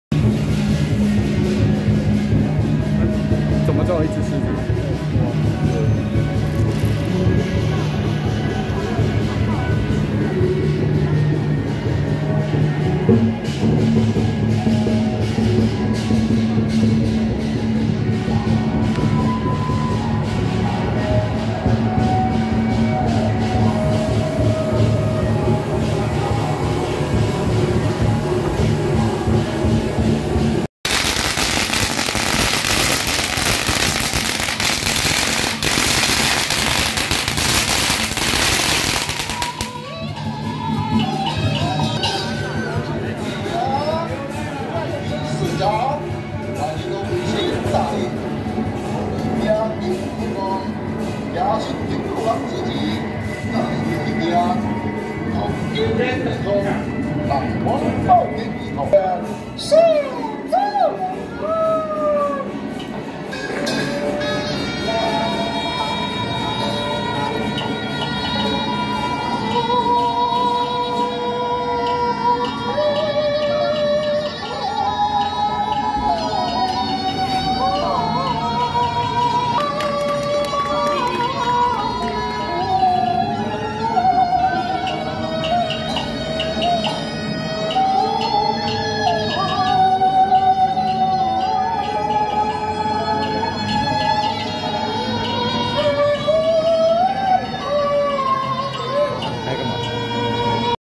【傳統漢服欣賞(中華漢服文化創意發展協會)】~111士林神農宮恭迎天上聖母遶境
傳統漢服欣賞(中華漢服文化創意發展協會)